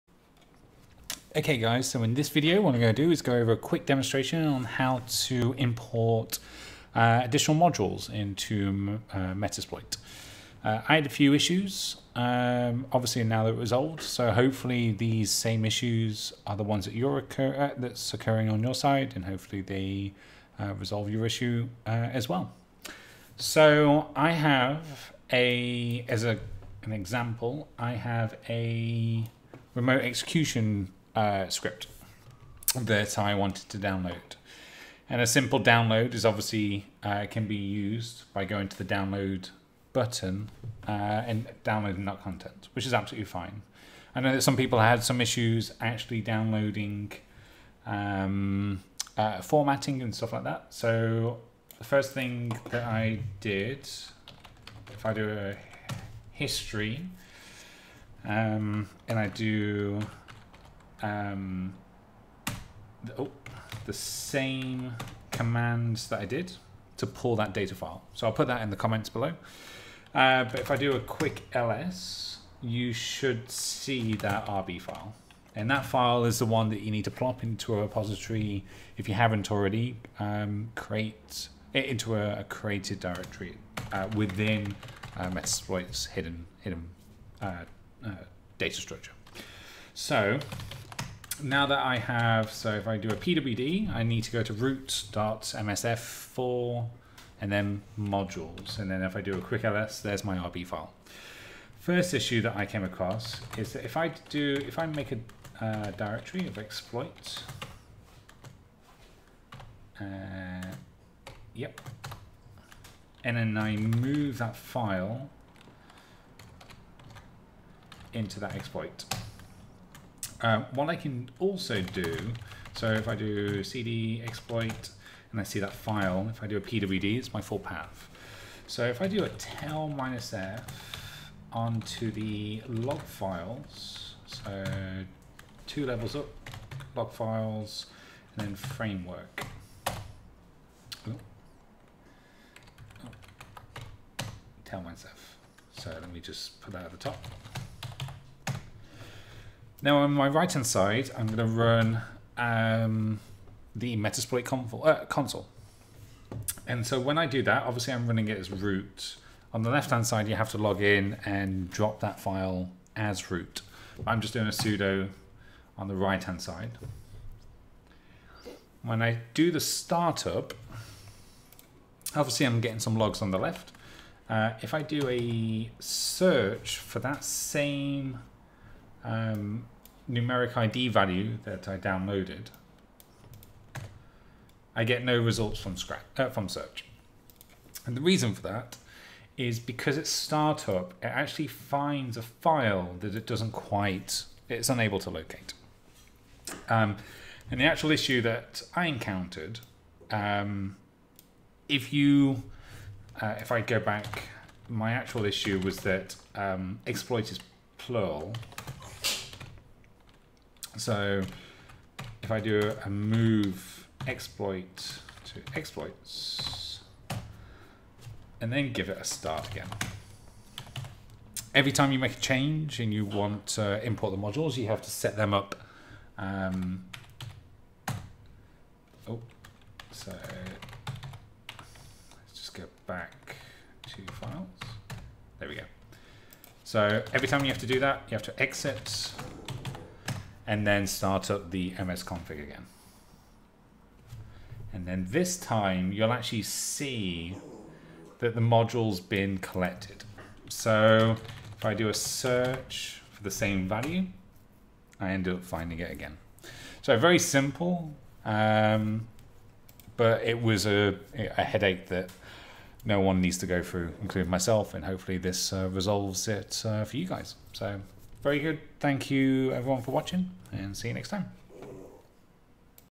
How to import modules into Metasploit - Resolving No results from search
Quick video on how to import new modules into Metasploit and thus resolving the 'no results from search' issue

Some helpful commands used within the video